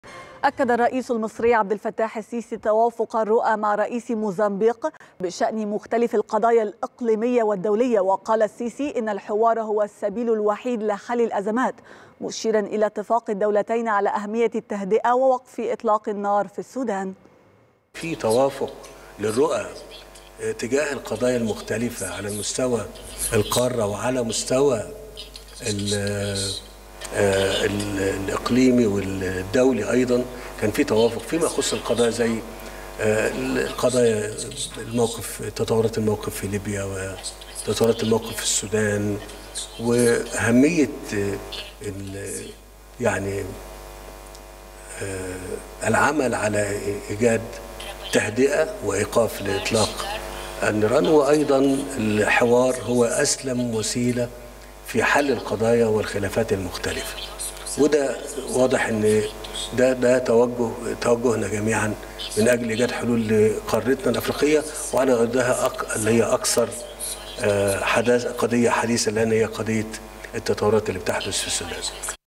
الرئيس المصري: الحوار هو السبيل الوحيد لحل أزمات المنطقة
9/6/2023